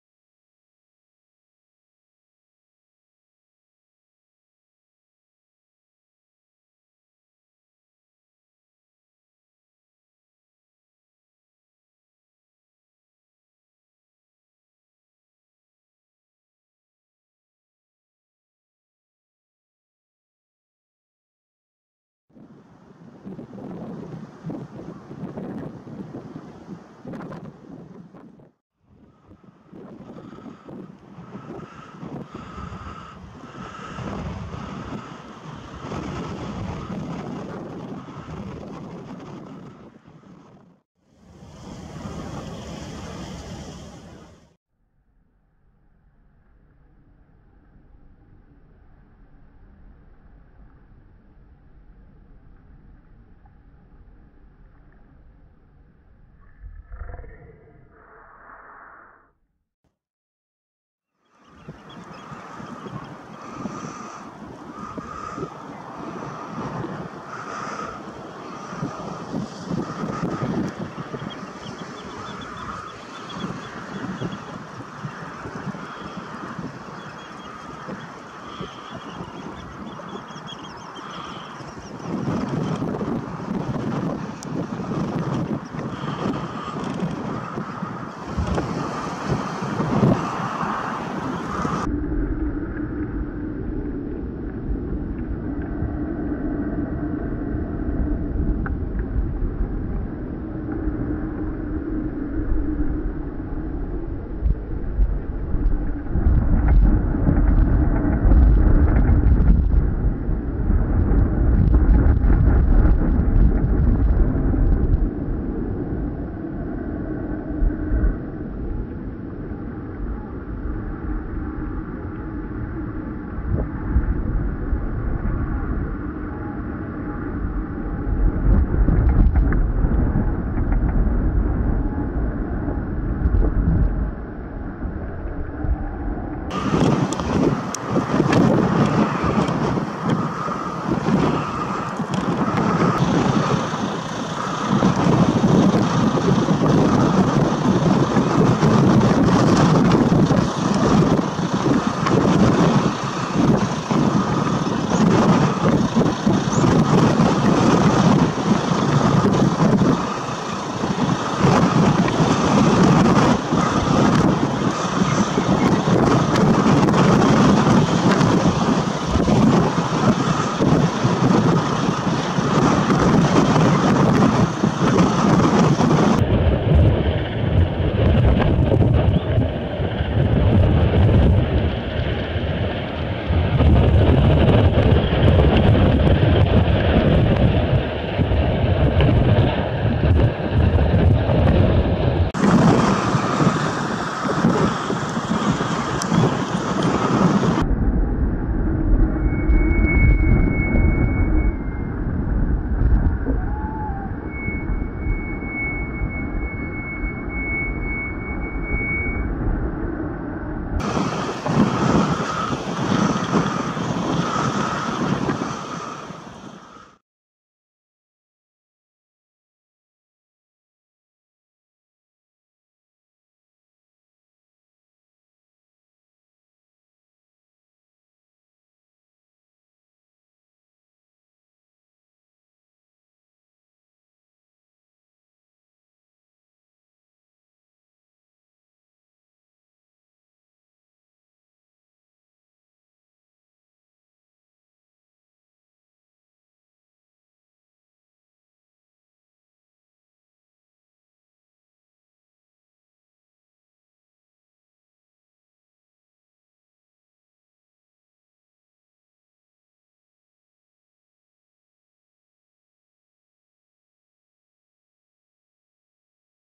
Red-throated Divers Yawning flying flapping / Roodkeelduikers gapend en uitslaand (Gavia Stellata)
Red-throated divers in march in front of the North-sea coast are sometimes gathered in pairs, waiting to leave for the big migration to the breeding grounds and waters. Footage of flying, yawning, preening and wing-flapping Red-throated Divers.
(Gavia Stellata)
Roodkeelduikers liggen voor de Brouwersdam soms verzameld in paartjes in Maart te wachten op de grote trek naar de broedgebieden. Beelden van vliegende, gapende, poetsende en uitslaande roodkeelduikers